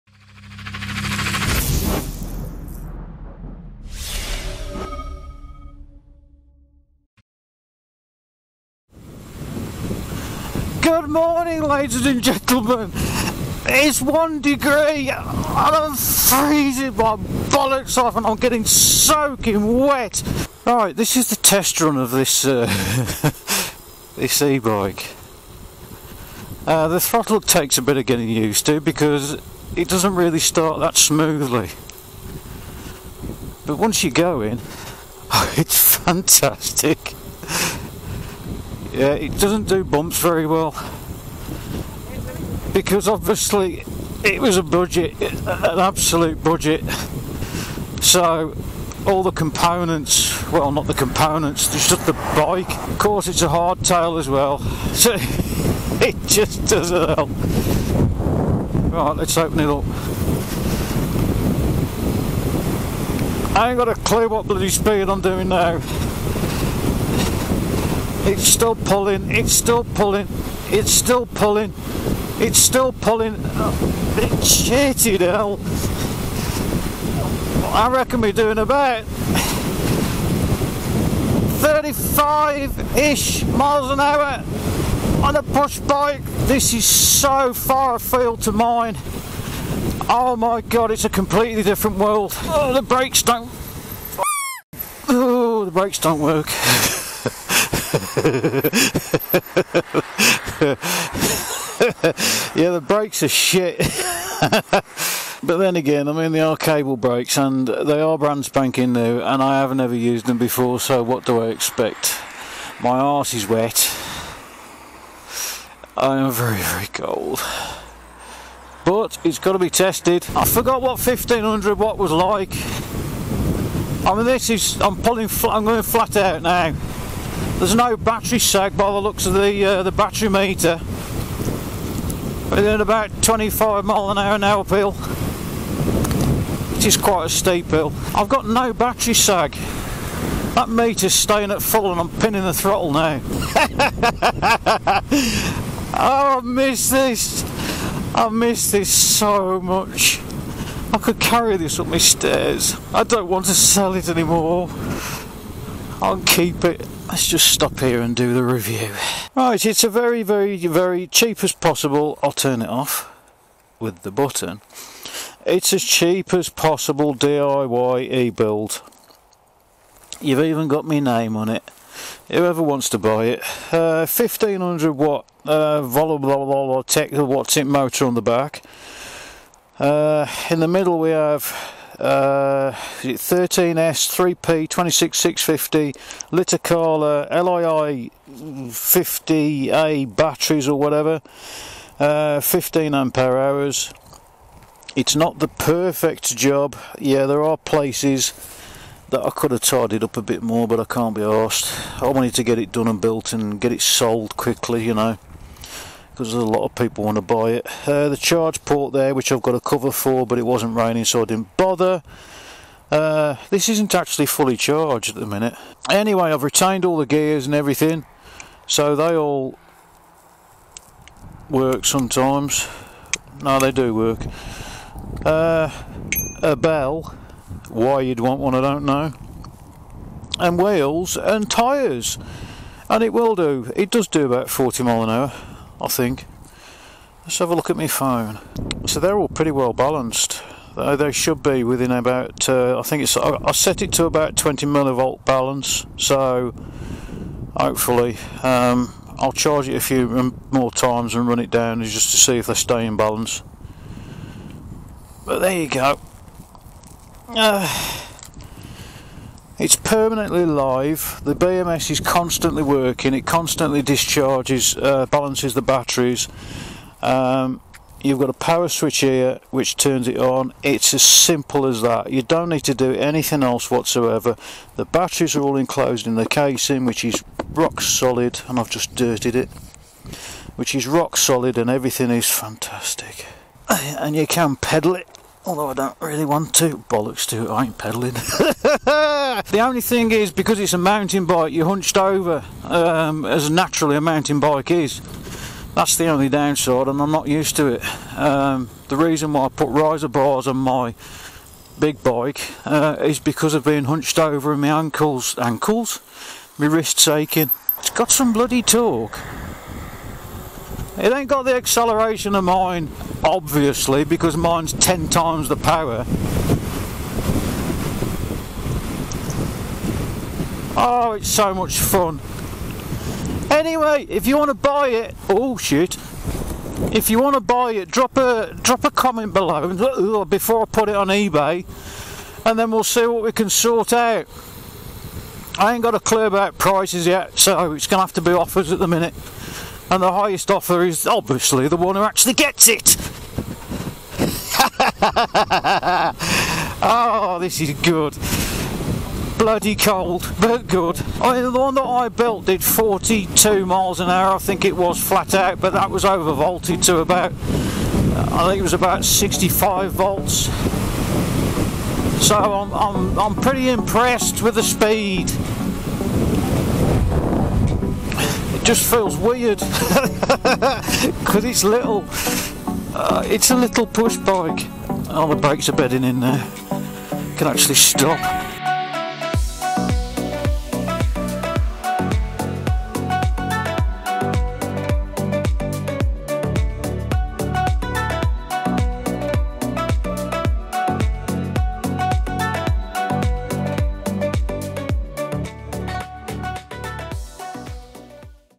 Budget 1500w DIY eBike build part 5 - The maiden run and it's now for sale!
You get to see some very 'juicy' and uncut bits that I daren't show here. Also, you'll get my support if you support me!

For anyone who is interested, I've listed it on my forum.

e-Bike parts used so far:

Violamart 1500W 48V Electric Bicycle Conversion Kit

Cross FXT500 cycle

Batteries (LiitoKala Lii-50A) are available from AliExpress

Nickel strip (pure nickel)

Active balancers - I'm not listing because they're crap (in my experience)

Real filament 1.75mm PETG black

The BMS can be bought here (obviously, just search for the one you need):
NOTE: They're not compatible with VBMS